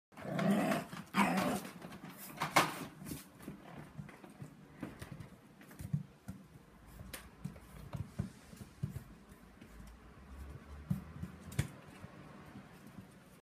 Is this Jack Russel or English bulldog
6 week old English bulldog in the middle of night